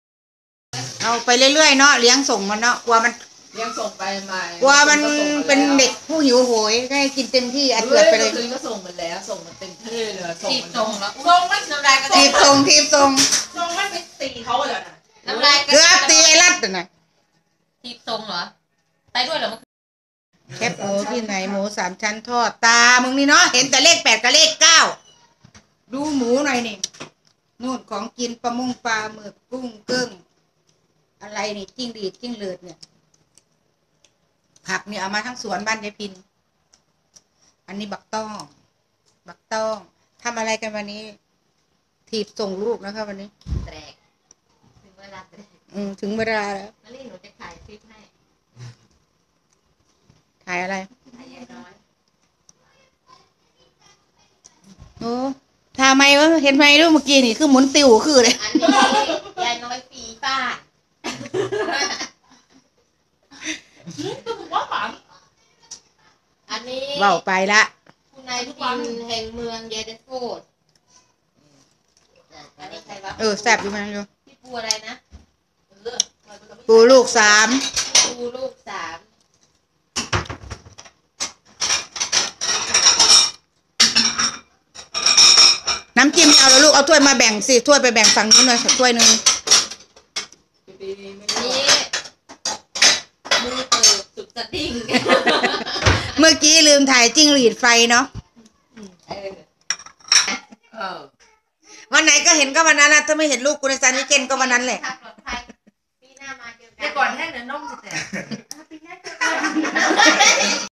พาลูกมากินข้าวบ้านเพื่อนก่อนกลับไทย อย่ากอดแรงเดียวนมแตก555
พาลูกมากินข้าวบ้านเพื่อนก่อนกลับไทย อย่ากอดแรงเดียวนมแตก555 เก็บตกความสุขที่เหลืออยู่